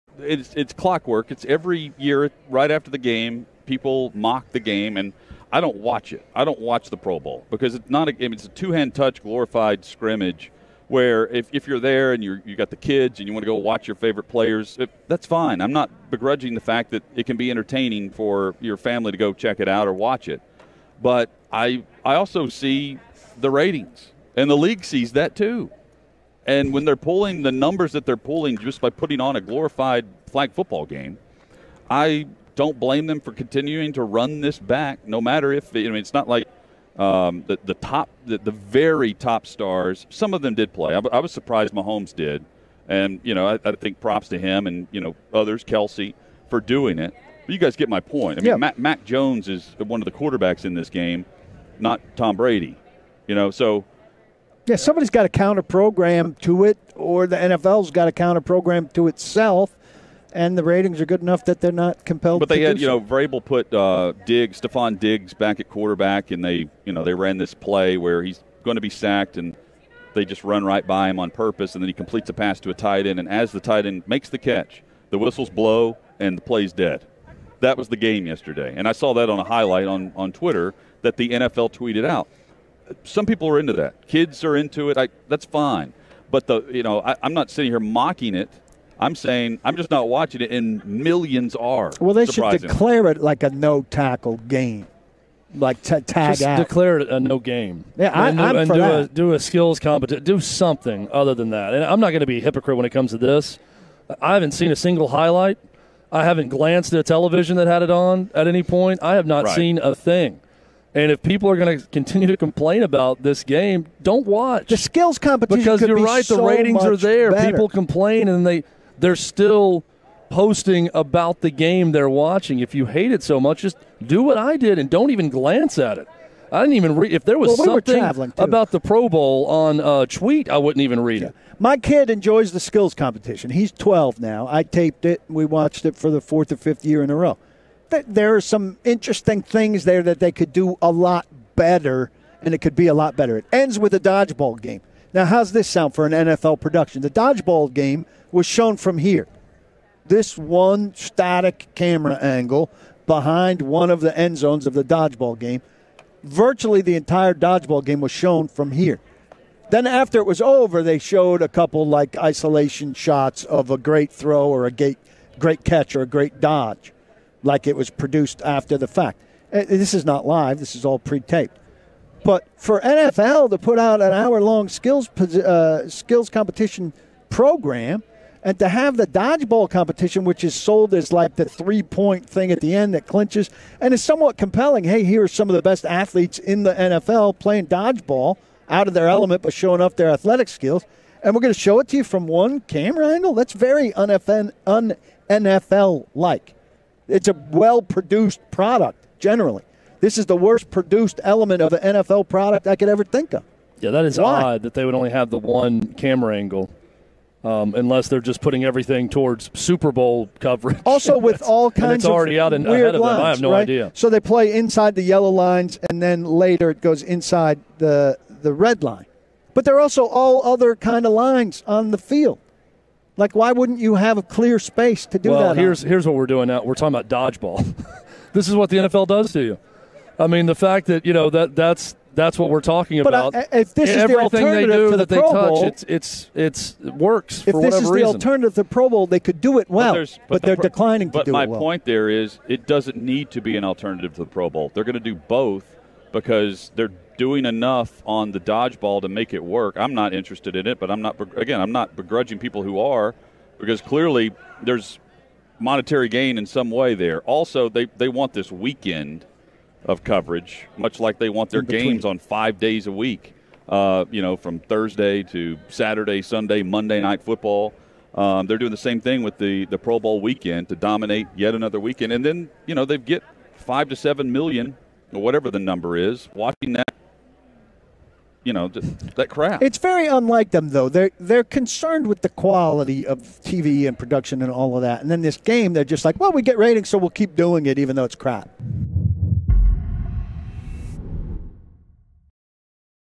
A Surprising MILLIONS Tune Into Pro Bowl 2022
When they’re pulling the numbers that they’re pulling just by putting on a glorified flag football game, I don’t blame them for continuing to run this back.